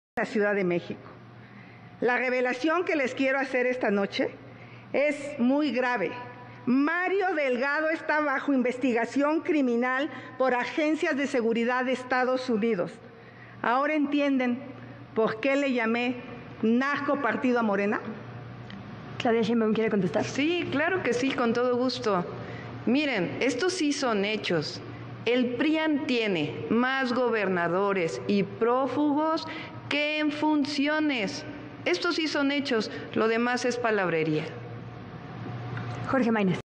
Xochitl Galvez asegura que en Estados Unidos hay una investigación contra el dirigente de Morena
Suben los ataques en el debate. Xochitl Galvez asegura que en Estados Unidos hay una investigación contra el dirigente de Morena, Mario Delgado. En cambio, Claudiashein recuerda a los gobernadores de la oposición que estuvieron bajo proceso penal.